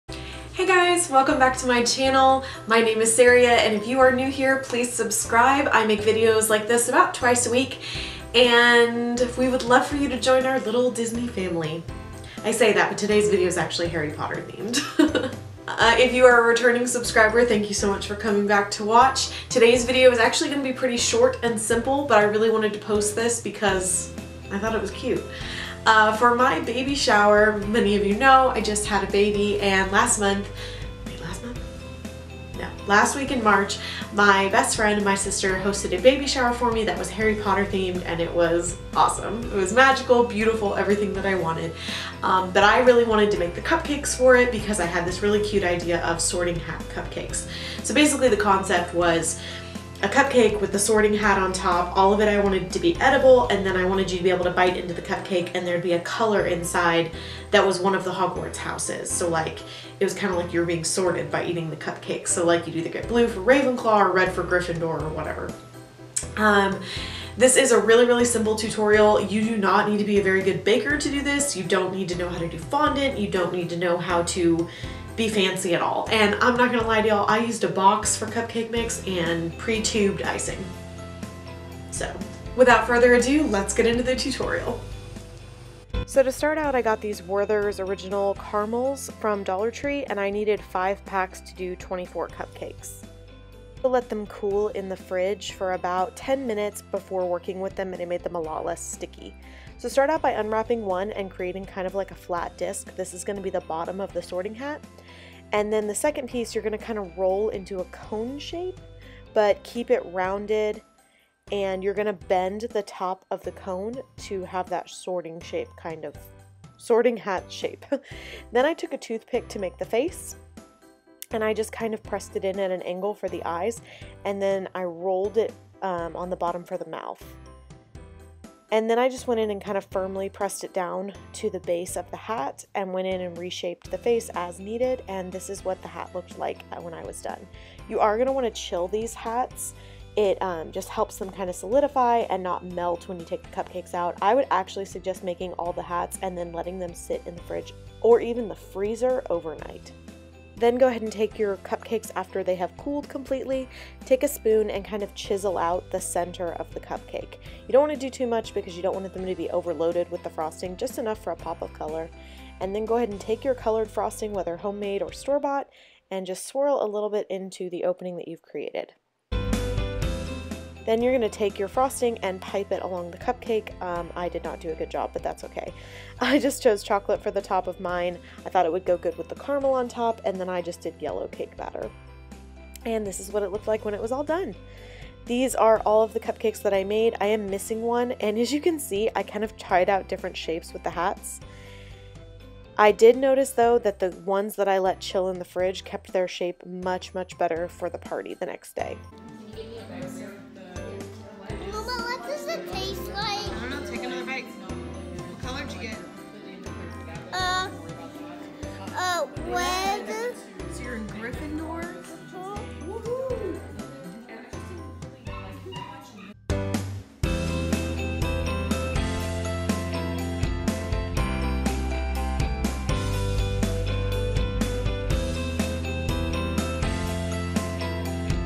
SUPER EASY HARRY POTTER SORTING HAT CUPCAKES
These are SOOOO easy anyone can make them! No fondant!

Supplies
Cake Mix (1 box)
Frosting (Blue, Red, Green, Yellow you won't need much of each)
Another frosting for the top of the cupcakes
Caramels (5 $1 Werther's SOFT Caramel bags)
Cupcake Liners
Spoon